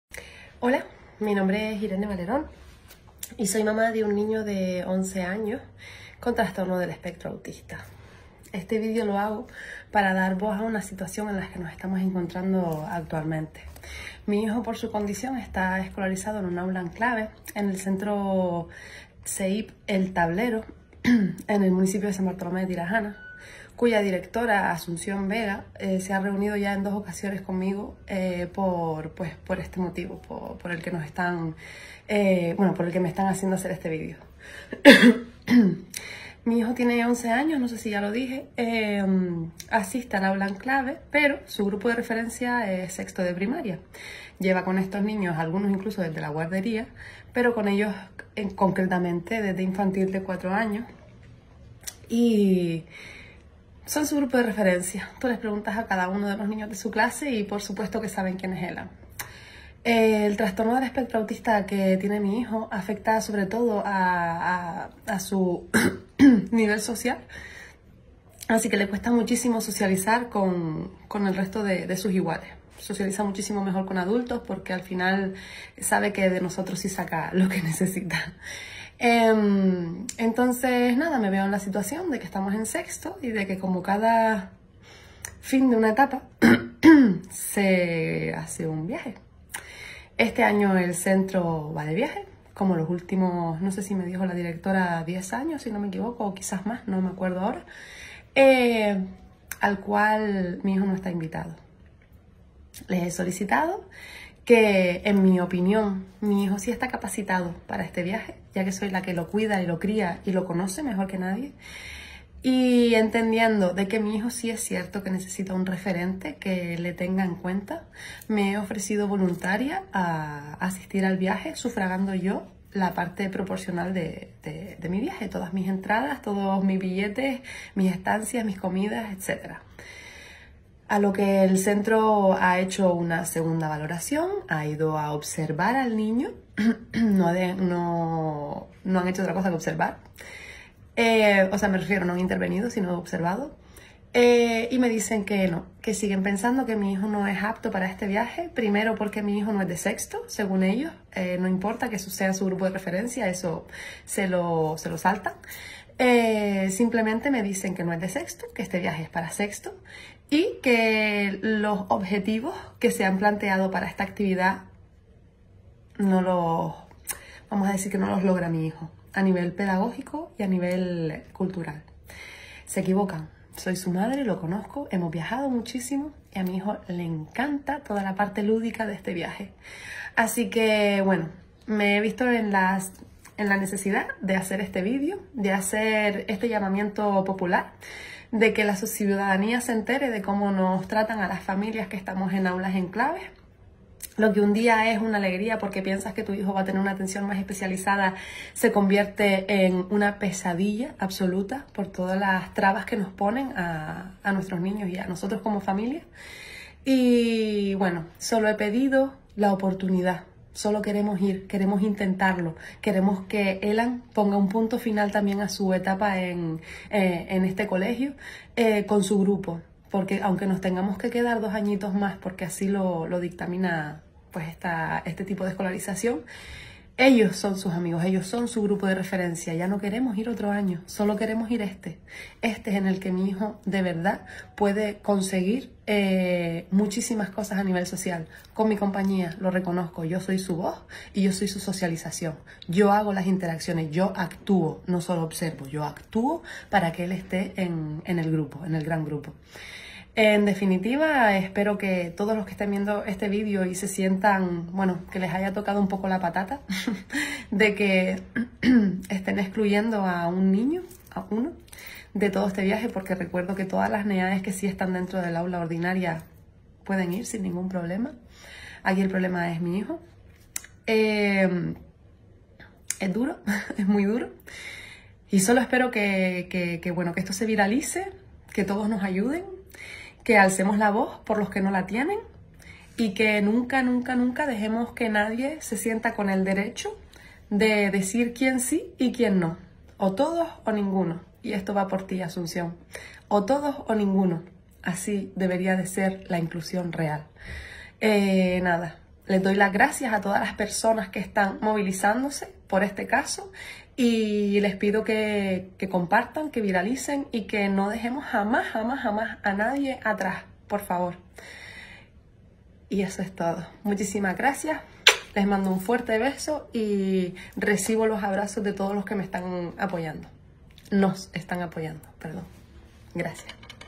La lucha de una madre por la " inclusión real" de su hijo en un colegio del Tablero
Elan es un niño de 11 años que asiste al aula en clave del colegio CEIP El Tablero. Y como todos sus compañeros de 6º de primaria, curso de referencia al que pertenece, tiene la ilusión de realizar el viaje de fin de curso.
Pero Elan tiene un transtorno del espectro autista. Esto hace que le cueste socializar con otros niños, por lo que su madre para que pueda asistir al viaje de fin de curso con sus compañeros, con los que comparte varias clases al día desde los 4 años, se ha ofrecido a acompañar a su hijo al viaje pagando sus gastos de su bolsillo.

El centro no se lo permite alegando que su hijo no cumple los objetivos que se han planteado para esta actividad ni a nivel pedagógico ni a nivel cultural. La pregunta seria si el resto de sus compañeros cumplen esos objetivos y cuales son. ¿No seria un buen objetivo enseñar a los niños a no discriminar a un compañero por ser diferente...o mejor dicho especial?.

Su madre está dispuesta a ser su voz para ayudarle a socializar con sus compañeros. Esto significaría un gran paso para Elan pero también un gran aprendizaje de la vida para sus compañeros.